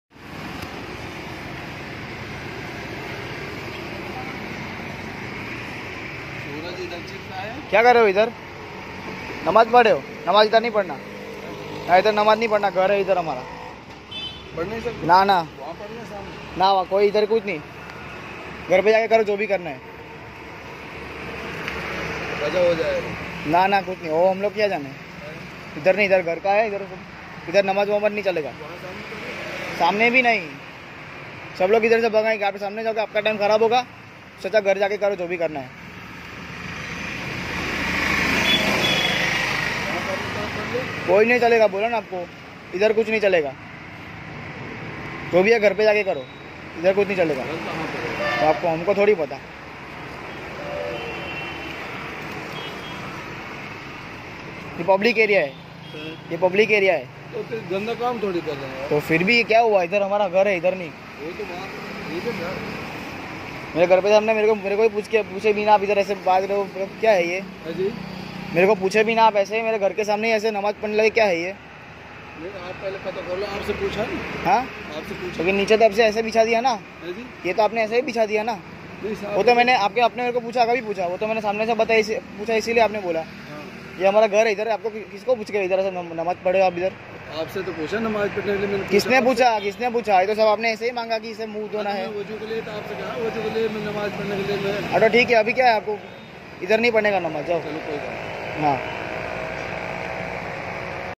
Ek Gareeb MUSALMAN ko Footpath par NAMAZ Padne se Rokagaya, Namaz sirf 5 Minute ki Hoti hai Bhai
Aaj ke is daur meh Kuch Hiindu Bhai log Aapni Zameen dehraha hai Musalmano ko Namaz ke liye

Ek Hogao saab iss meh Saab ki Balai Hai, Beshak, Jai Hind